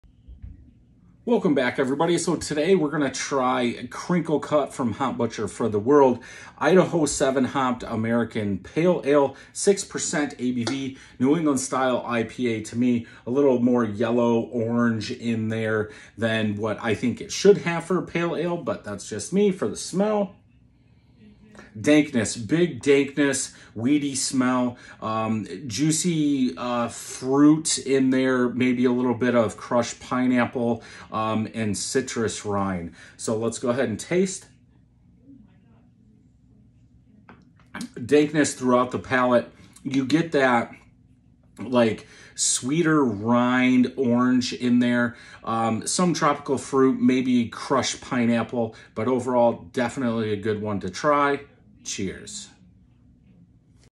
Hop Butcher Crinkle Cut
Hop Butcher Crinkle Cut is a New England IPA by style. Idaho 7 hopped American Pale Ale. This craft beer is 6% ABV. In this craft beer review, we will take a look at the color, smell, and taste.